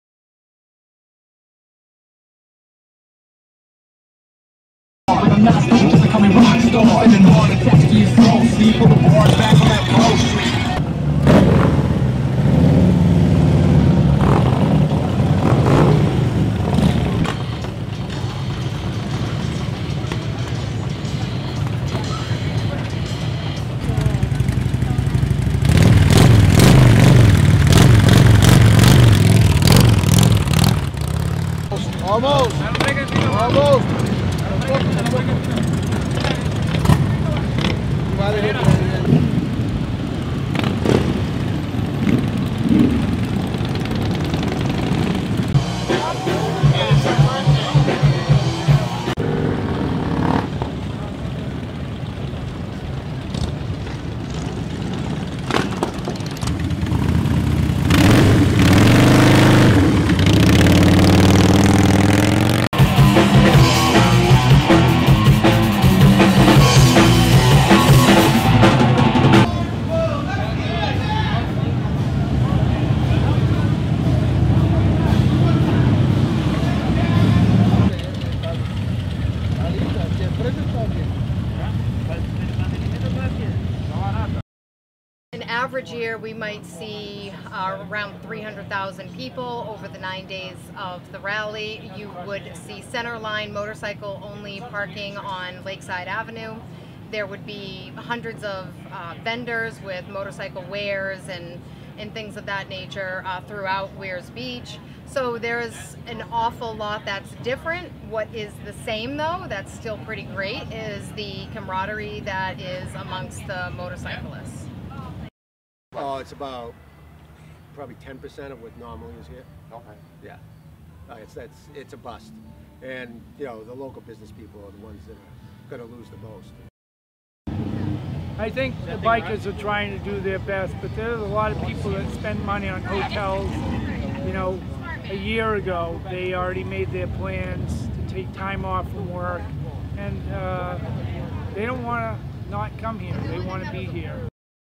Laconia Bike Week draws less riders than previous years amid COVID 19 pandemic
The 97th annual Laconia Bike Week celebration in New Hampshire, which usually draws hundreds of thousands of bikers lining the streets, sees a significantly lower attendance than previous years amid the COVID-19 pandemic after it was delayed from June to late August.